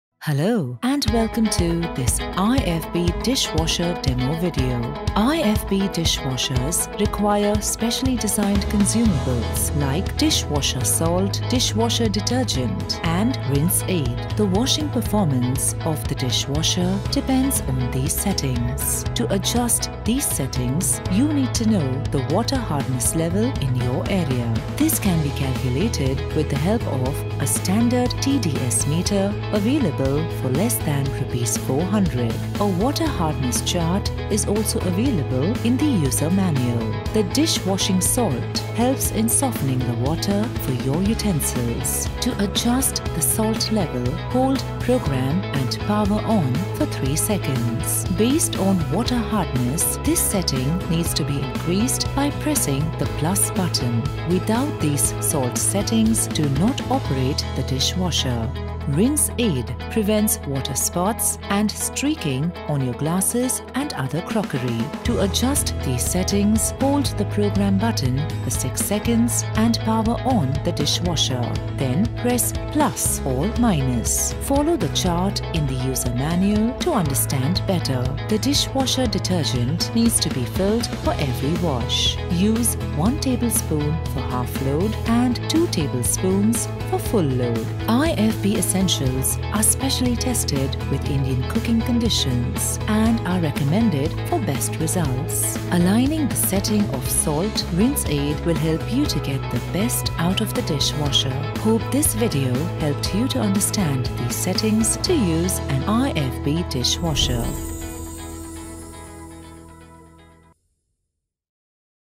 IFB dishwasher detergent, salt and rinse aid setting
Get the best out of an IFB Dishwasher by using the proper settings for dishwashing detergent, salt and rinse aid.